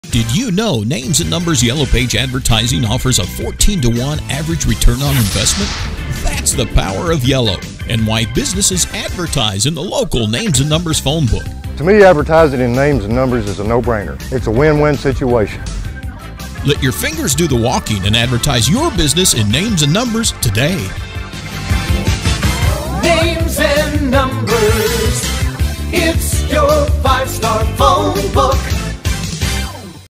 Business Review - Browns Collision in Northwest Arkansas Reviews 2015 Names and Numbers Yellow Pages
Brown's Collision Center in Northwest Arkansas provides review of 2015 Names and Numbers print, online and mobile Yellow Pages.

According to Association of Directory Publishers (ADP), Yellow Page advertising offers an average 14 to 1 return on investment (ROI).